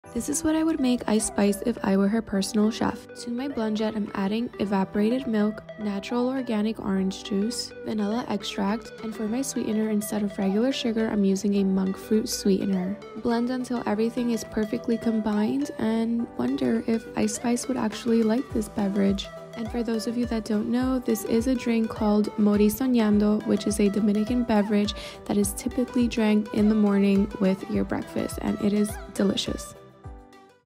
Morir Soñando BlendJet Recipe
Whipping up a zesty delight! Blending magic with the BlendJet to create the perfect 'Morir Soñando' – a dreamy citrus concoction that's pure sunshine in a glass! Do you think @IceSpice would like this Morir Soñando?

Servings: 2

INGREDIENTS
4 cups evaporated milk
2 1/2 cups orange juice
1 Tbsp vanilla extract
3/4 cup monk fruit sweetener

DIRECTIONS
1. Blend everything in your BlendJet and enjoy!